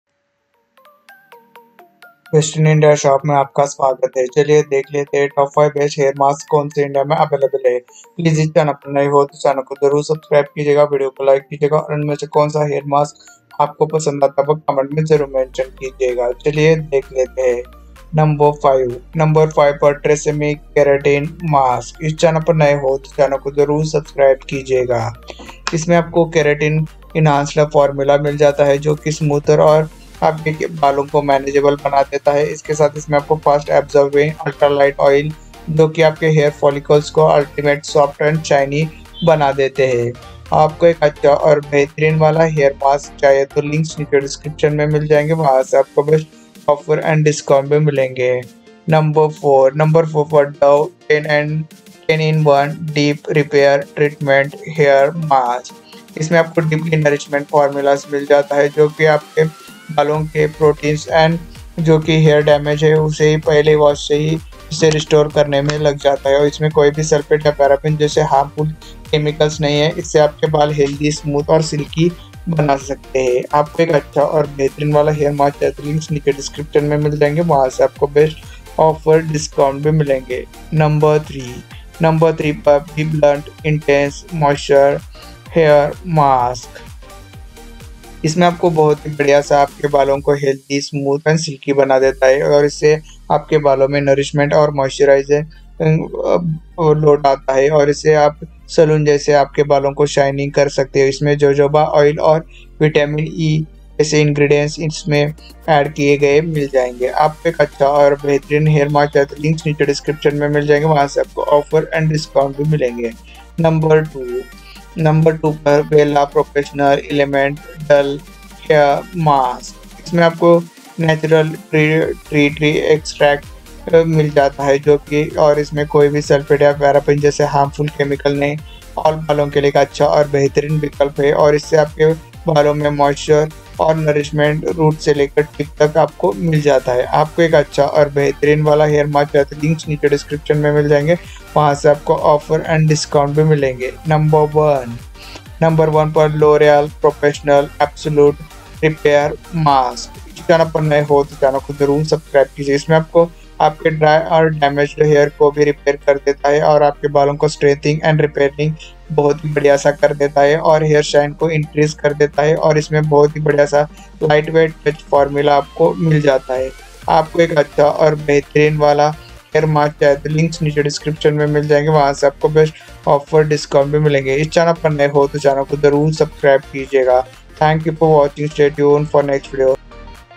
Best Hair Mask in India 2024 | सबसे अच्छा हेयर मास्क
Best Hair Mask in India | सबसे अच्छा हेयर मास्क

List of Top 5 Best Hair Mask in India 2024

1. L'Oréal Professionnel Absolut Repair Mask :

2. WELLA Professionals Elements Dull Hair Mask

3. BBLUNT Intense Moisture Hair Mask

4. Dove 10 in 1 Deep Repair Treatment Hair Mask

5. TRESemmé Keratin Mask

Checkout the list of Top 5 Best Hair Mask in India with Reviews.
Follow us on::

Which brand hair mask is best in India?
Which mask is best for hair?
What is the very best hair mask?